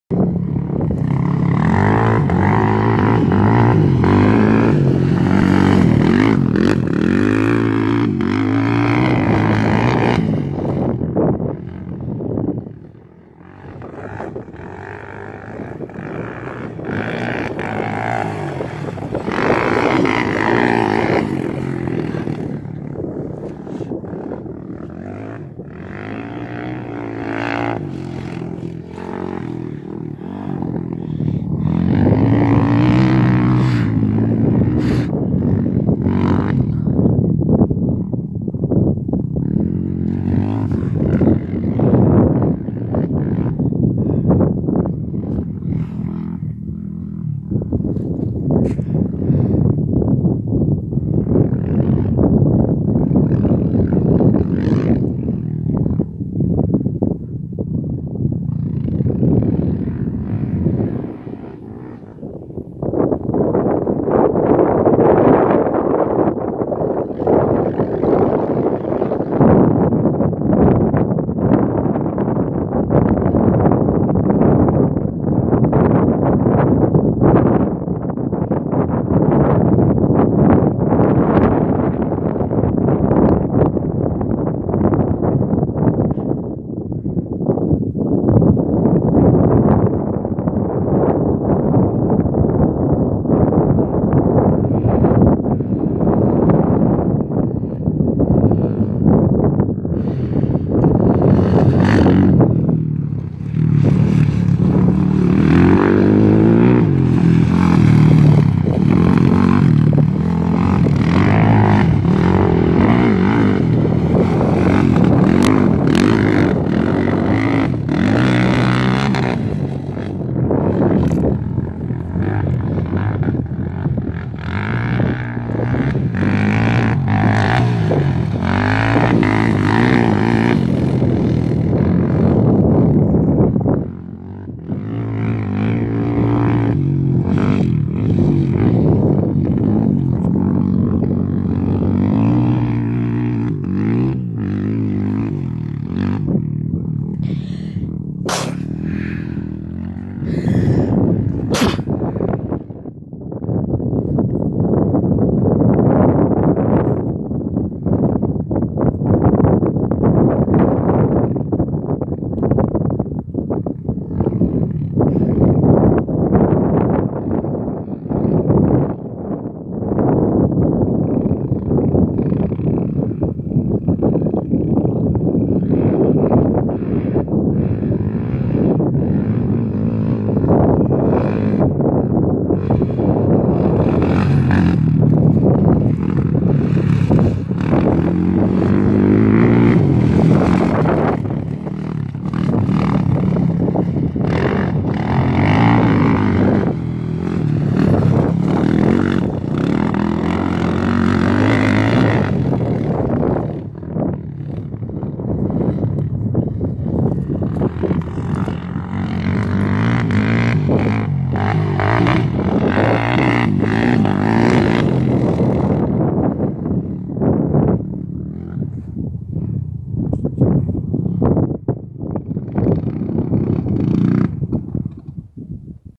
Searchlight NV riding TRACK Practice FUN HONDA TRX450R
Riding at the track like usual always practicing and just having fun on the Honda 2004 TRX450R kicker.TRACK FUN